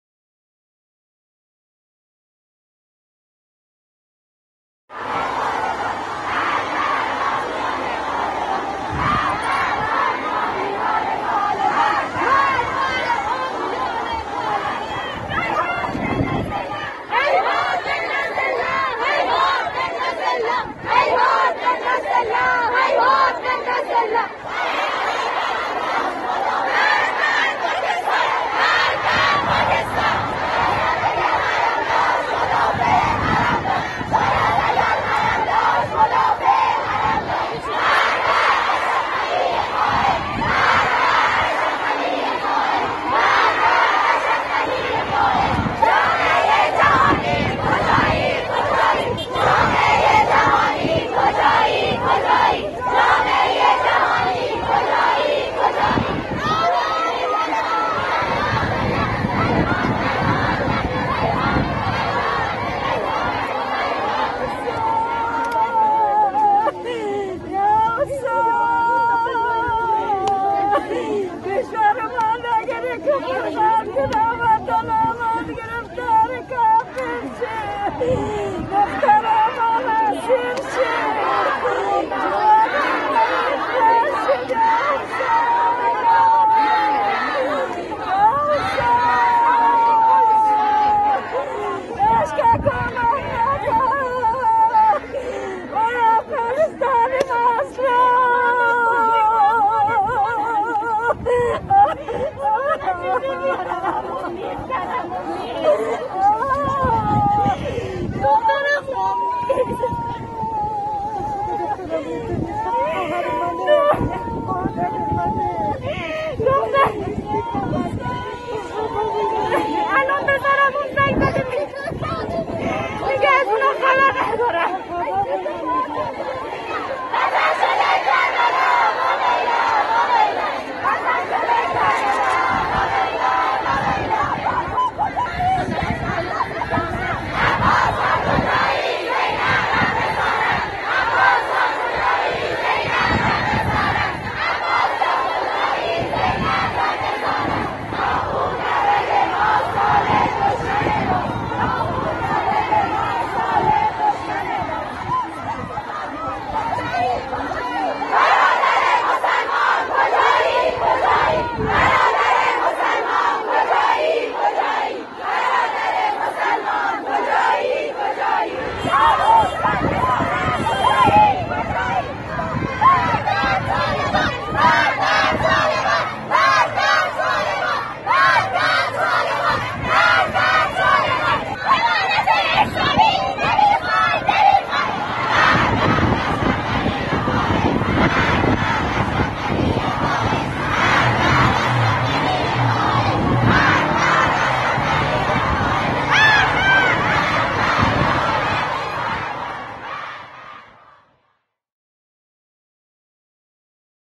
تجمع افغانستانی‌های مقیم قم در پی تحولات کابل
در پی اتفاق‌ها و تحولات اخیر در افغانستان، بویژه تصرف کابل توسط نیروهای طالبان، جمعی از مردم افغانستانی‌های مقیم شهر قم، صبح دوشنبه (۲۵ مرداد) در این شهر تجمع کردند و خواستار حمایت جهانی از افغانستان و شهروندان آن شدند.